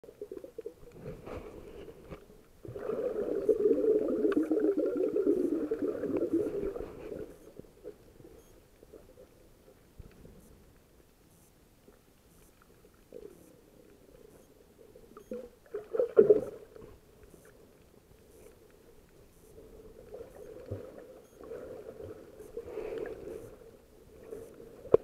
Free Diving Off N2 Paddle Board, Baby Remora
While free diving off my North 2 Boards 10'6" Adventure Paddle Board out of Boynton Beach inlet. Came across this friendly little baby Remora who followed me around for a while! Crystal clear South Florida water is amazing! Get out and explore.